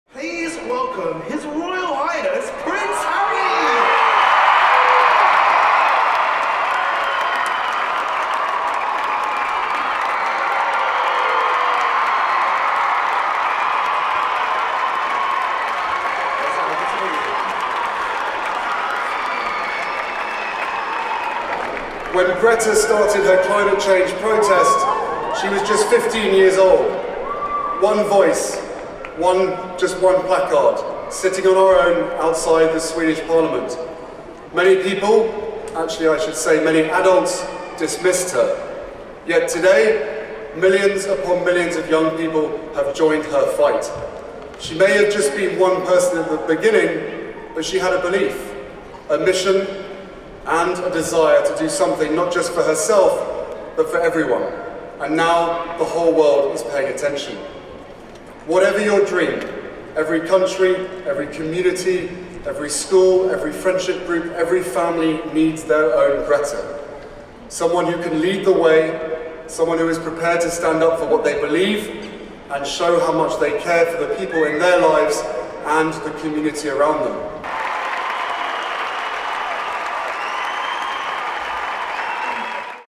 The Duke of Sussex Praises Greta Thunberg in Emotional Speech
Prince Harry praised climate activist Greta Thunberg when he took to the stage at an awards ceremony in London on Sunday.

The duke said she had been "one voice, with just one placard, sitting on her own outside the Swedish parliament" when she began her climate campaign, when she was dismissed by many adults.

Prince Harry was speaking at the OnSide Youth Zones awards, which aim to recognise the achievements of young people, volunteers and staff at the UK charity.